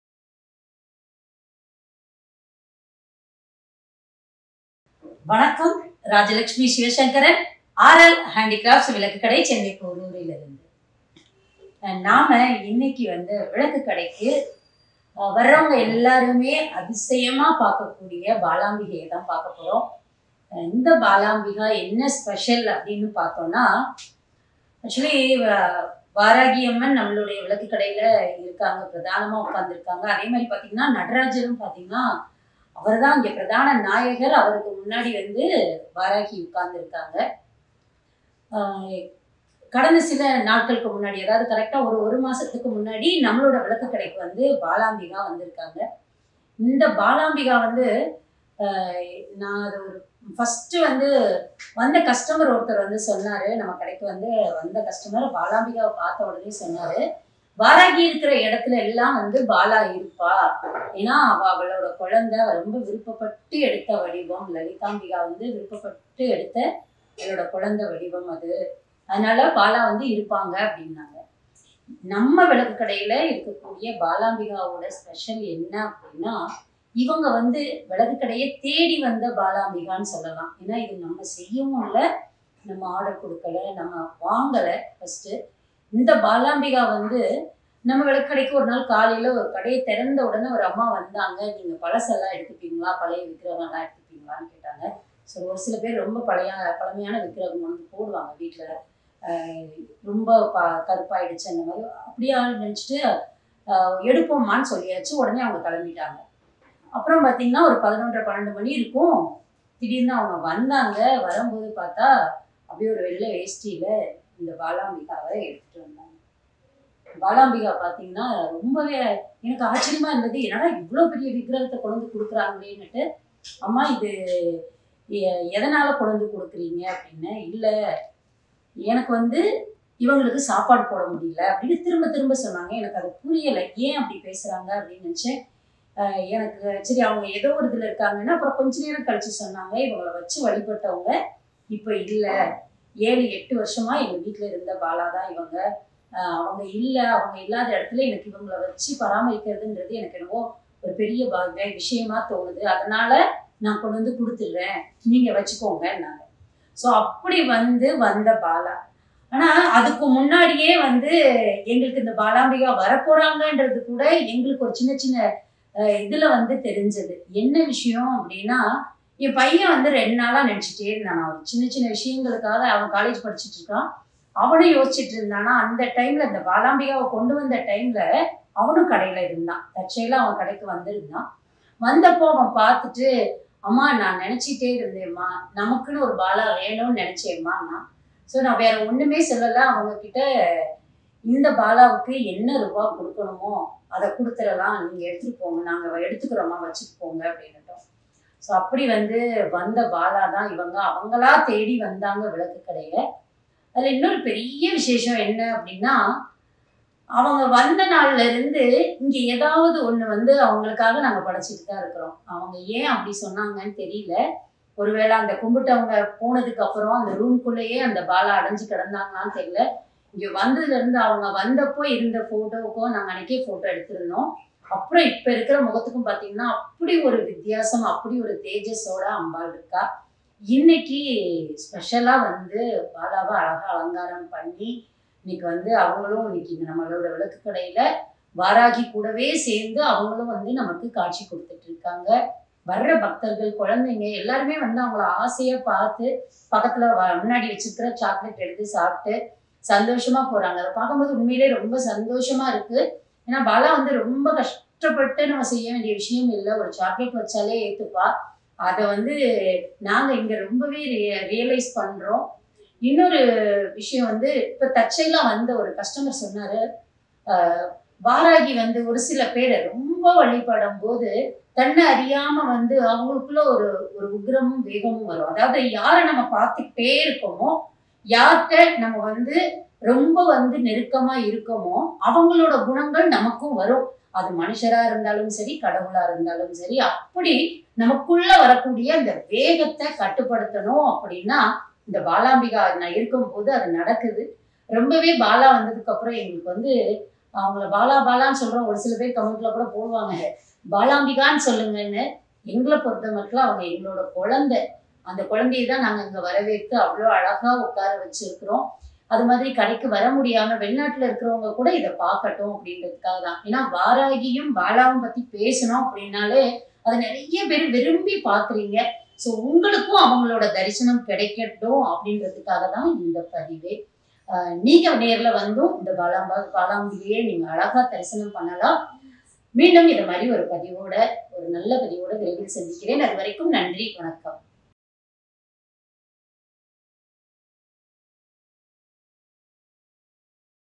அருள் பாலிக்கும் பாலாம்பிகையின் தரிசனம் | Vilakkukadai | RLHandicrafts
கண்ணையும் கருத்தையும் கவரும் வண்ணம்
விளக்கு கடைக்கு தானாகவே வந்திருந்து அருள் பாலிக்கும் பாலாம்பிகை

பார்த்துக் கொண்டே இருக்கலாம்